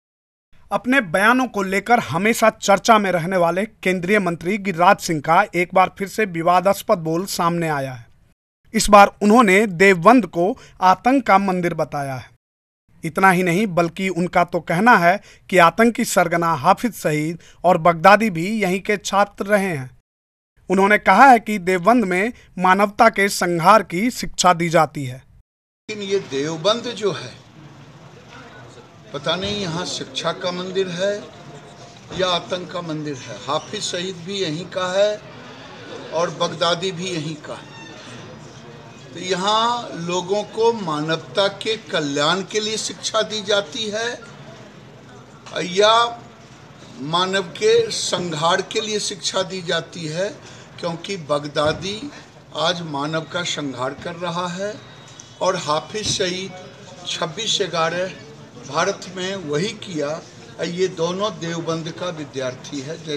केंद्रीय मंत्री गिरिराज सिंह के विवादित बोल, देवबंद को बताया आतंक का मंदिर
सहारनपुर : अपने बयानों को लेकर हमेशा चर्चा में रहने वाले केंद्रीय मंत्री गिरिराज सिंह का एक बार फिर से विवादास्पद बयान सामने आया है. इस बार उन्होंने देवबंद को आतंक का मंदिर बताया है. इतना ही नहीं उन्होंने ये भी कहा कि आतंकी सरगना हाफिज सईद और बगदादी भी यहीं के छात्र रहे हैं. उन्होंने कहा कि देवबंद में मानवता के संहार की शिक्षा दी जाती है.